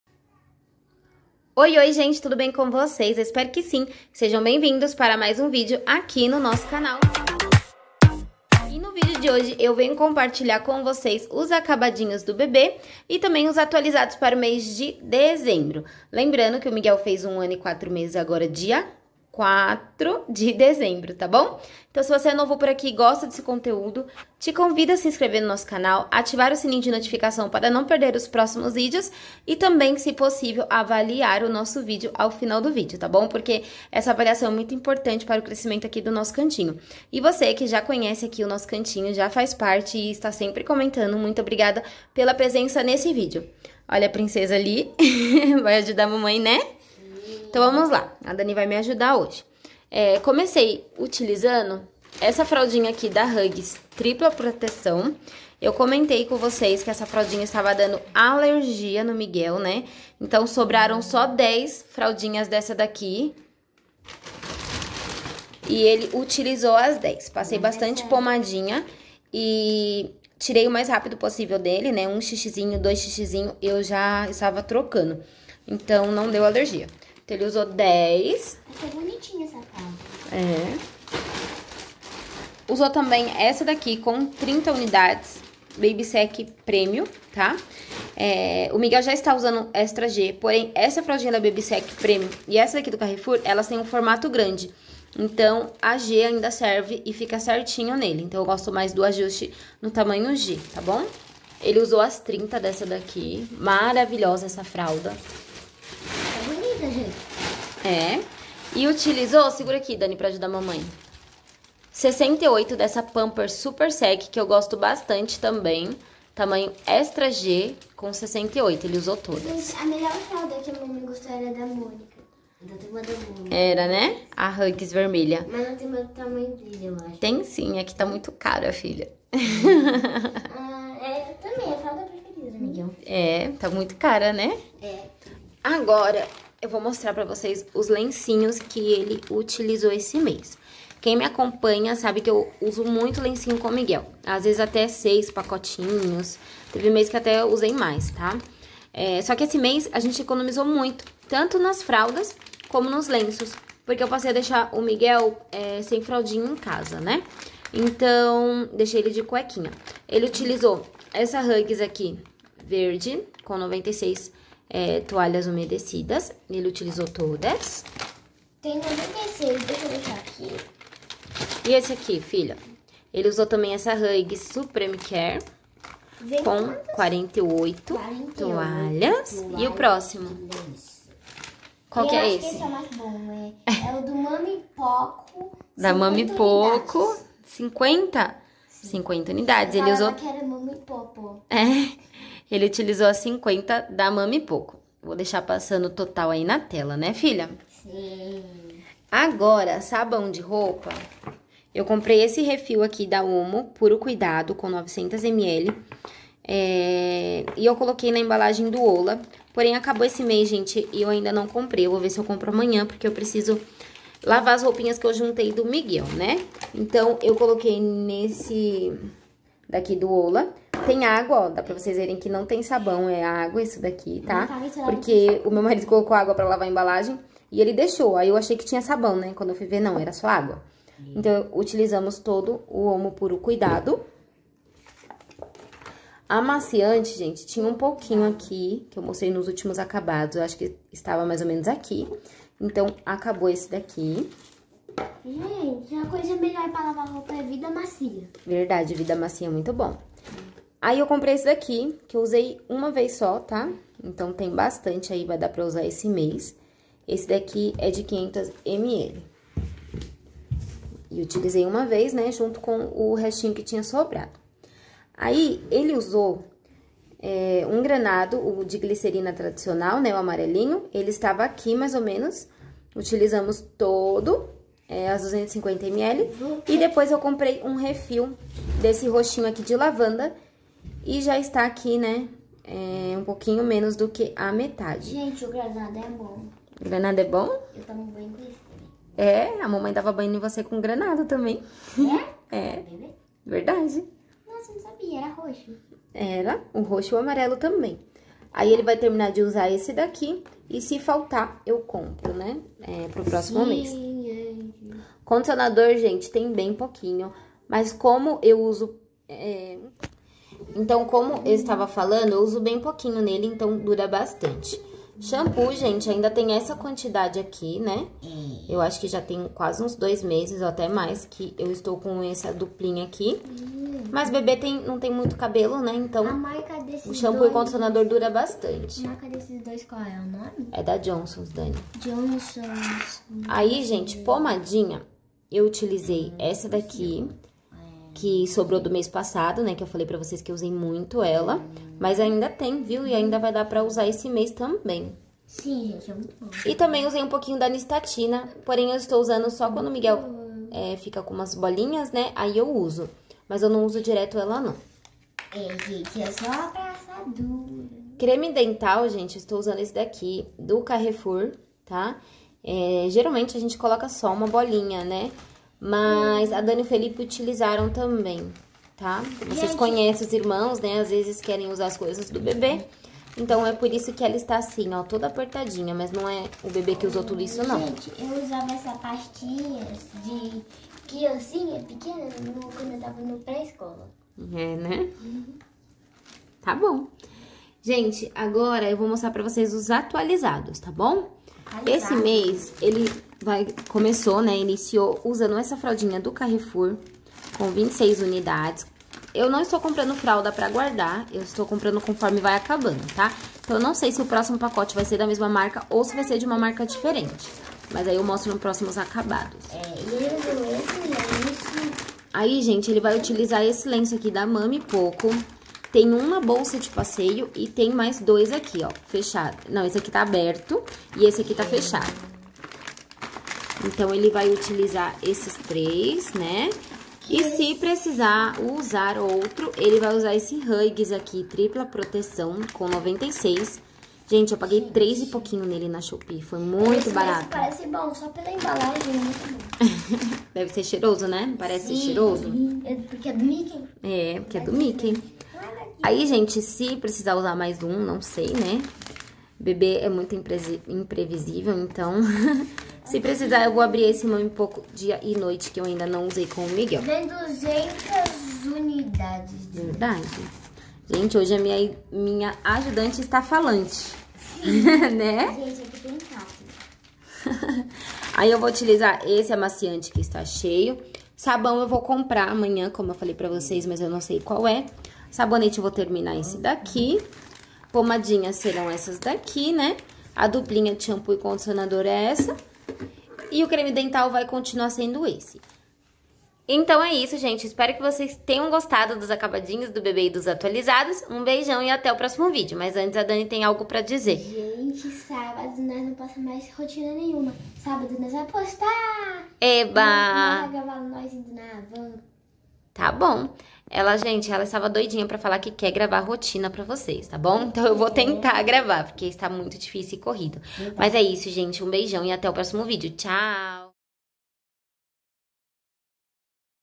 ACABADOS DO BEBÊ | 1 ANO E 4 MESES | ATUALIZADOS DE DEZEMBRO.
Olá, espero que estejam bem!
Hoje venho compartilhar mais um vídeo de acabadinhos do bebê.
Vem conferir?